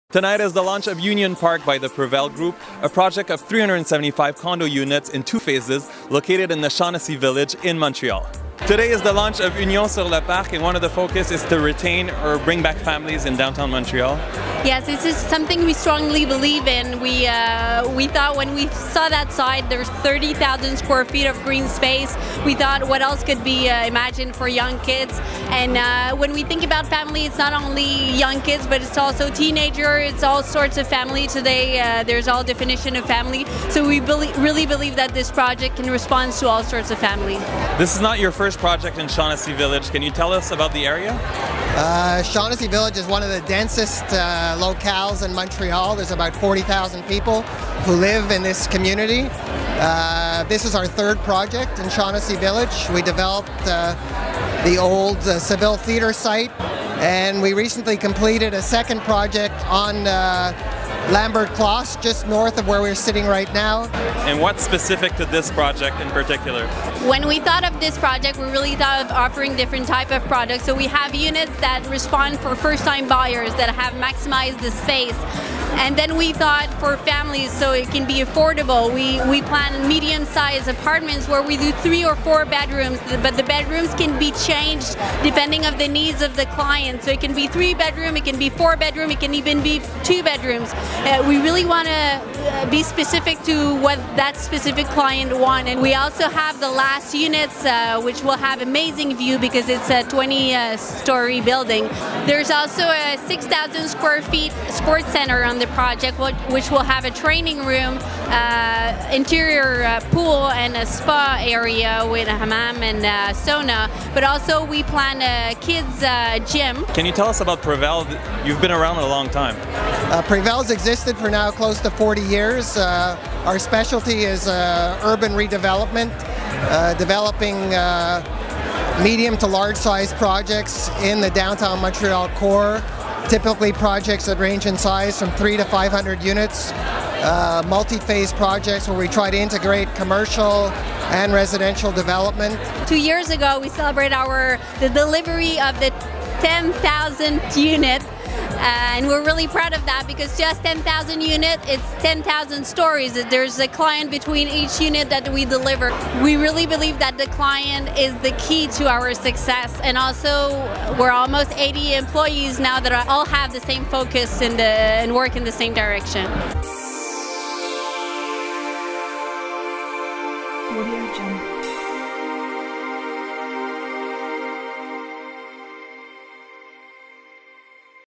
Prével launches Union Park condo project in Montreal | Montreal.TV
Union Park is your chance to own a piece of one of downtown Montreal’s rare greenspaces. Located in Shaughnessy Village, recently renamed the Grand Gardens, this condominium project is part of Montreal’s green corridor, extending from Mount Royal to the Lachine Canal. It’s an exceptional living environment brought to life by a multicultural neighborhood in full transformational blossom. In this video report, Laurence Vincent and Jonathan Sigler from Prével.

Reporter: Martin Rego.